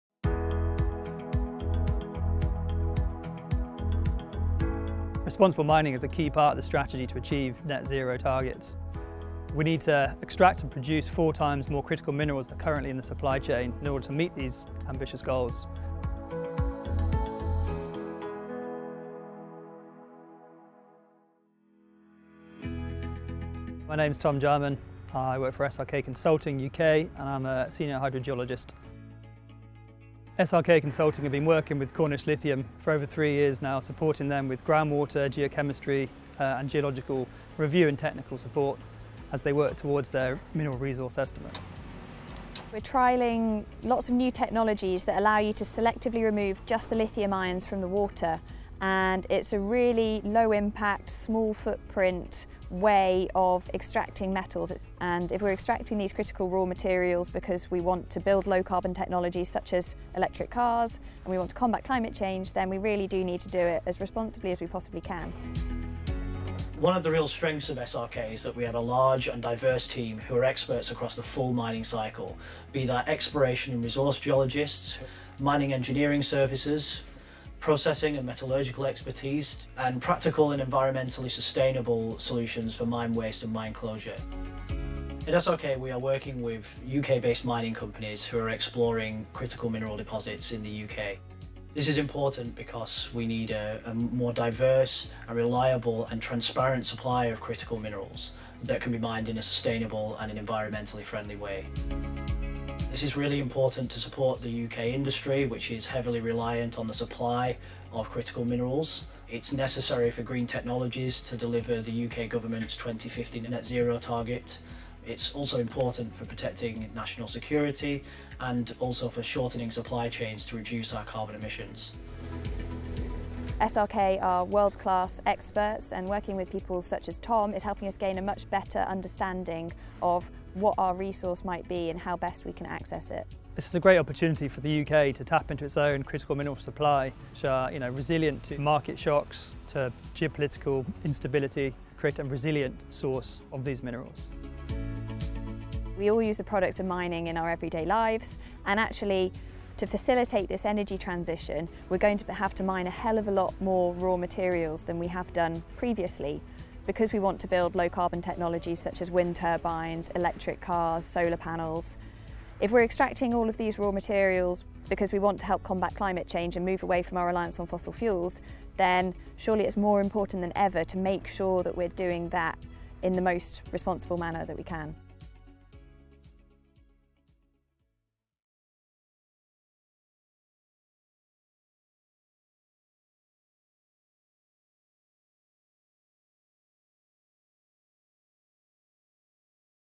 Responsibility in Mining | SRK Consulting | Material Change
We can’t achieve net zero without mining critical minerals.

We need copper and aluminium to produce cleaner electricity. Lithium, cobalt and graphite are needed for wind turbines, batteries and EV charging.

Discover how SRK Consulting is working with UK based mining companies to responsibly source these critical minerals.